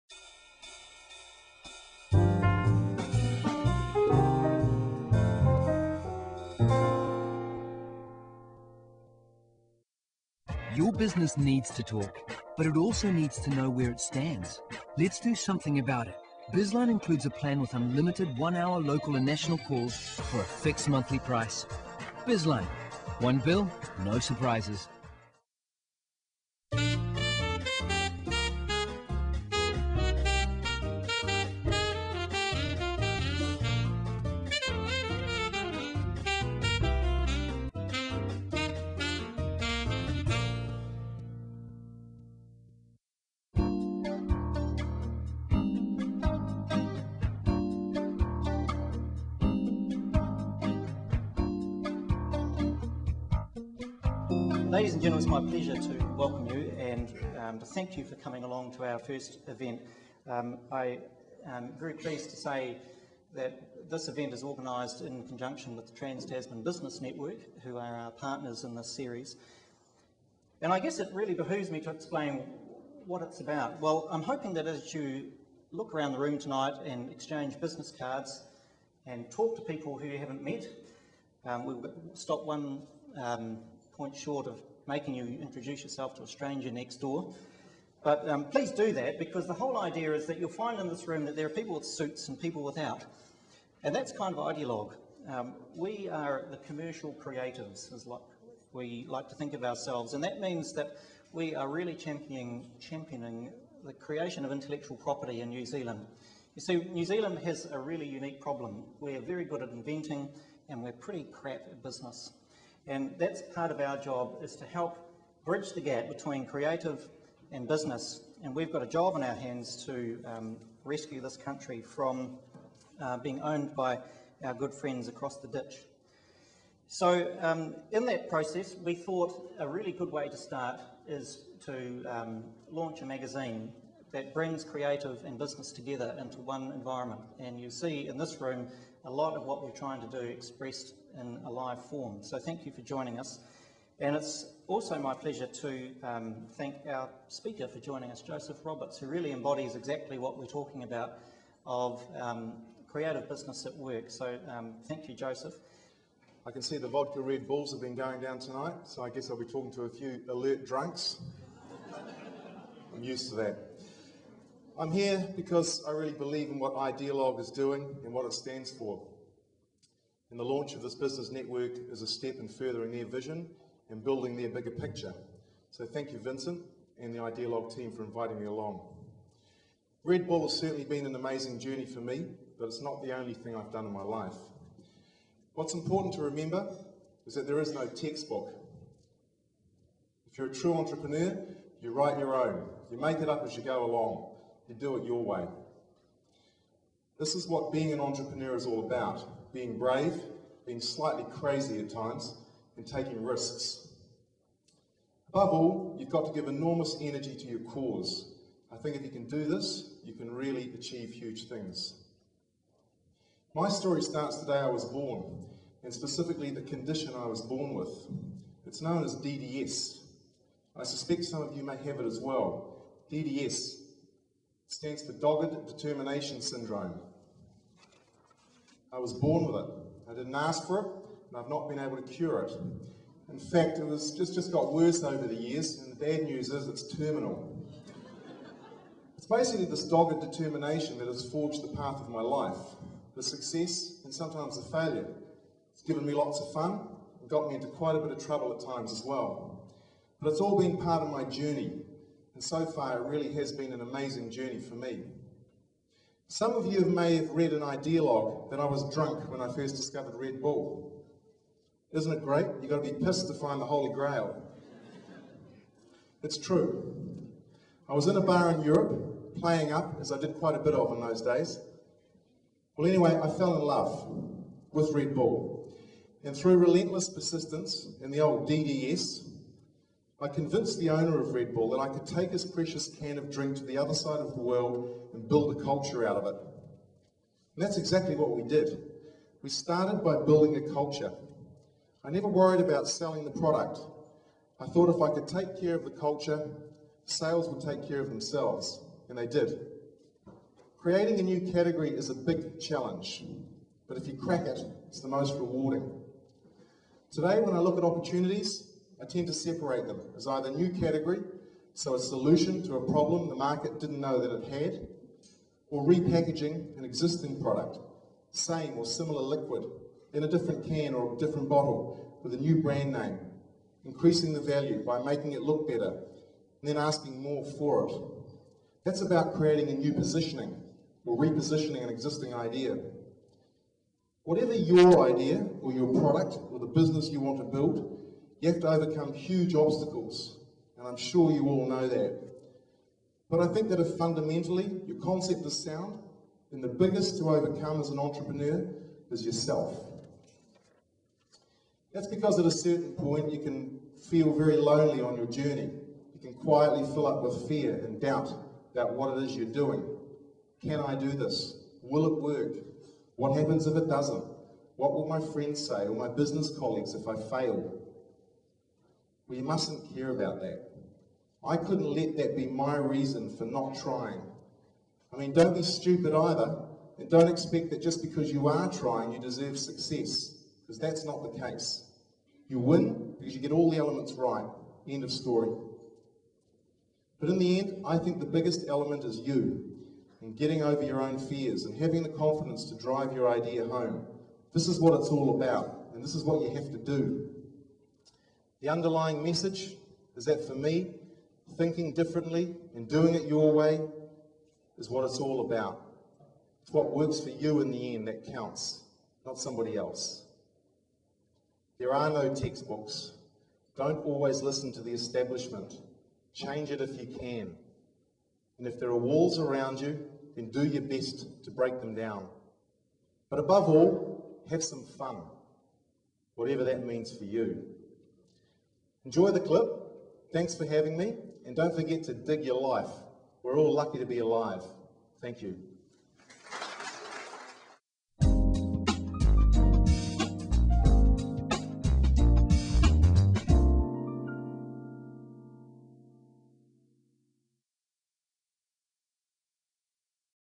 Josef Roberts @ AUT-Idealog Innovation Series
Josef Roberts ('the man who gave Red Bull wings') was the guest speaker at the inaugural AUT-Idealog Innovation Series at the Auckland Art Gallery.